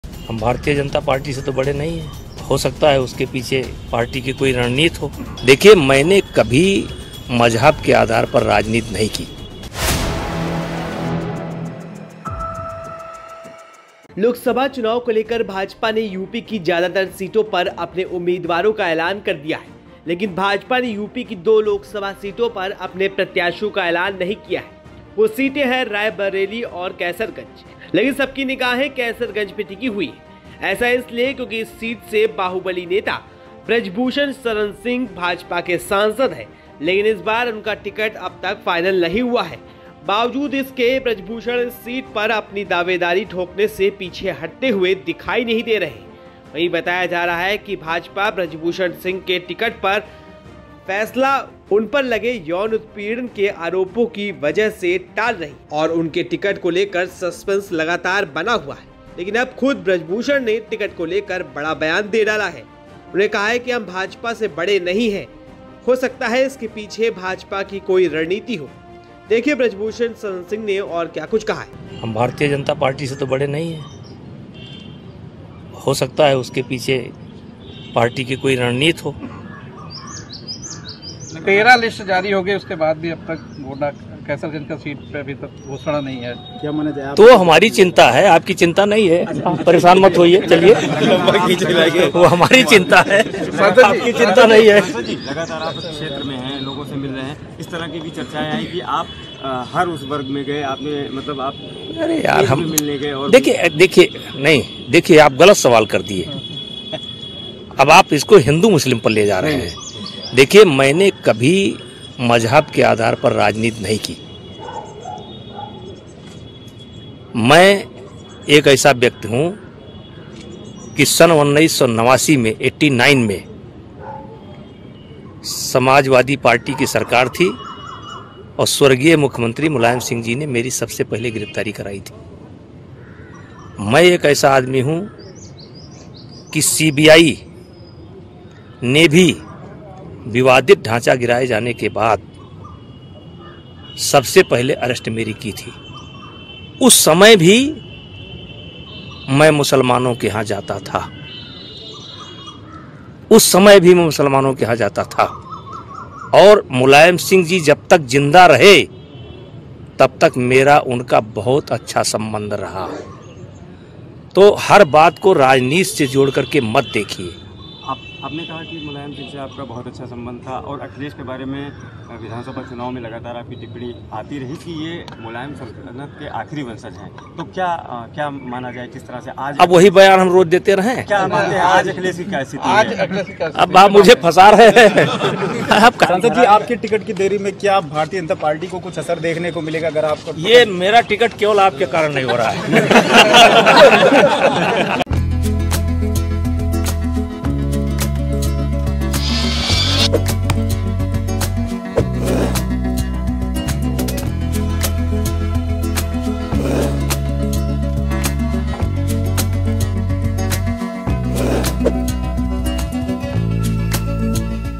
Brij Bhushan Sharan Singh ने खुद बता दी टिकट न मिलने में देरी की वजह ! | Lok Sabha Election 2024
लोकसभा चुनाव को लेकर भाजपा ने यूपी की ज्यादातर सीटों पर अपने उम्मीदवारों का ऐलान कर दिया है..  लेकिन भाजपा ने यूपी दो लोकसभा सीटों पर अपने प्रत्याशियों का ऐलान नहीं है.. वो सीटे हैं रायबरेली और कैंसरगंज.. लेकिन सबकी निगाहें कैसरगंज पर टिकी हुई है.. ऐसा इसलिए क्योंकि इस सीट से बाहुबली नेता बृजभूषण शरण सिंह भाजपा के सांसद हैं.. लेकिन इस बार उनका टिकट अब तक फाइनल नहीं हुआ है बावजूद इसके बृजभूषण सिंह इस सीट पर अपनी दावेदारी ठोंकने से पीछे हटते हुए दिखाई नहीं दे रहे हैं..